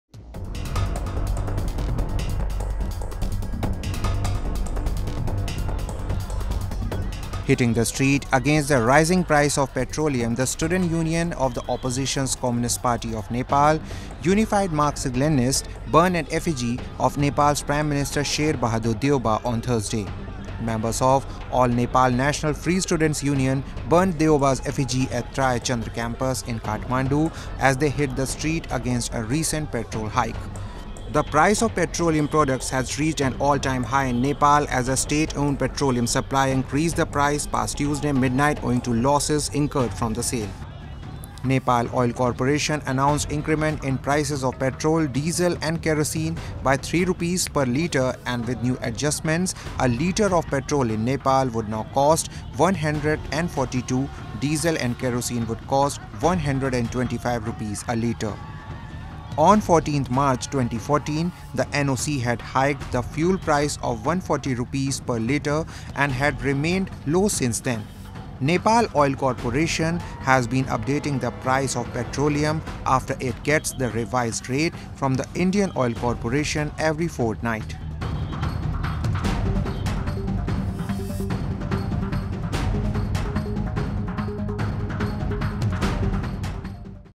Opposition burns effigy of Nepalese PM, urges govt to reduce petrol price
Kathmandu (Nepal), Jan 04 (ANI): Hitting the street against the rising price of petroleum, the student union of the opposition's Communist Party of Nepal (Unified Marxist-Leninist) burned an effigy of Nepal’s Prime Minister Sher Bahadur Deuba on Thursday. Members of the All Nepal National Free Students Union (ANNFSU) burnt Deuba's effigy at Tri-Chandra Campus in Kathmandu as they hit the street against a recent petro hike. The price of petroleum products has reached an all-time high in Nepal as the state-owned petroleum supplier increased the price on Tuesday midnight owing to losses incurring from the sales. Nepal Oil Corporation announced increment in prices of Petrol, Diesel and Kerosene by 3 rupees per litre and with new adjustments, a litre of petrol in Nepal would now cost 142, Diesel and Kerosene would cost 125 rupees a litre. On 14 March 2014, the NOC had hiked the fuel price to 140 rupees per litre and had remained low since then. Nepal Oil Corporation has been updating the price of petroleum after it gets the revised rate from Indian Oil Corporation every fortnight. (KP)